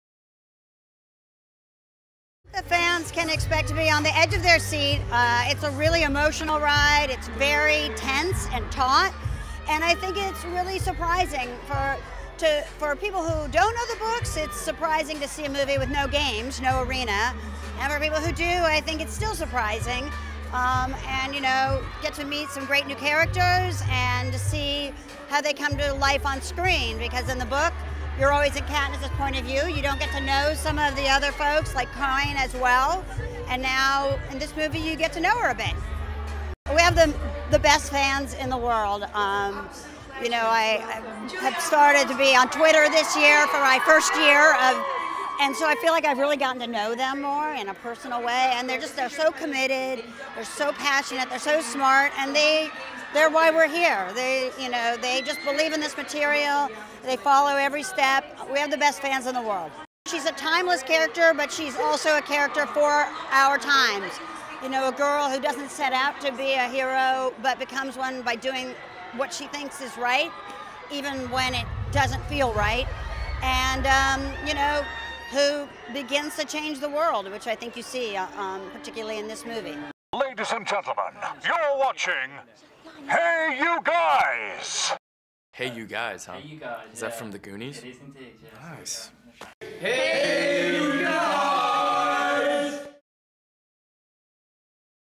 Producer Nina Jacobson Interview - The Hunger Games Mockingjay Part 1 Premiere
Producer Nina Jacobson is interviewed at the World Premiere of The Hunger Games Mockingjay in London.

The Premiere was attended by Jennifer Lawrence, Josh Hutcherson, Liam Hemsworth, Sam Claflin, Elizabeth Banks, Jena Malone, Julianne Moore, Stanley Tucci, Jeffery Wright, Natalie Dormer, Lorde, Director Francis Lawrence and Producers Nina Jacobson & Jon Kilik.